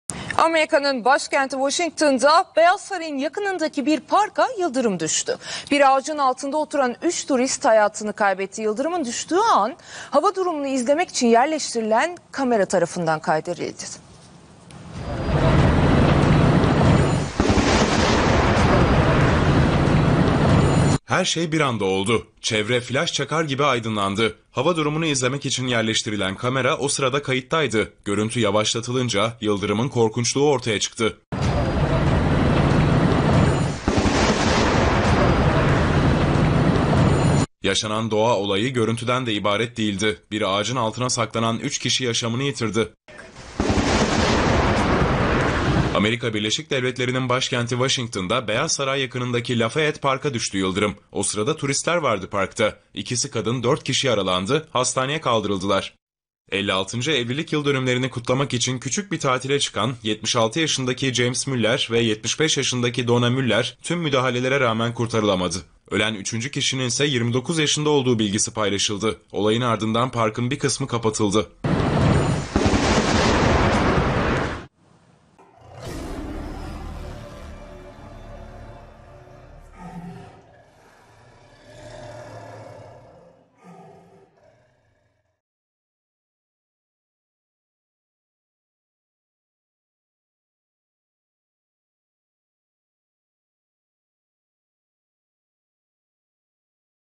Washington'daki Beyaz Saray'ın yakınında bulunan parka yıldırım düştü: 3 ölü
SON DAKİKA: Beyaz Saray yakınına yıldırım düştü: 3 ölü, 1 yaralı - Son Dakika Haberleri Beyaz Saray yakınına yıldırım düştü: 3 ölü, 1 yaralı Müller  ana haber